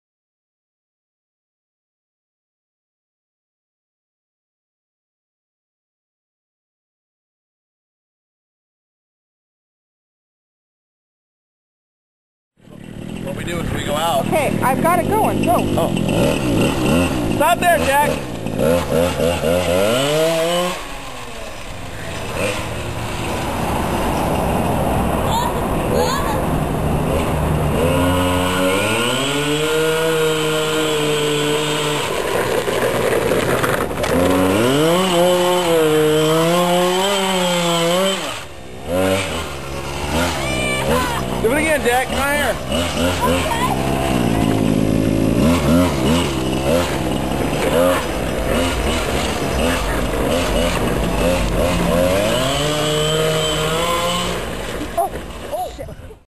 Jack VS RC Truck
Found this vid buried. I think Jack was about 5-6, Had a 1/5th scale gas RC truck and He had a Big Wheel. WHAT FUN!!!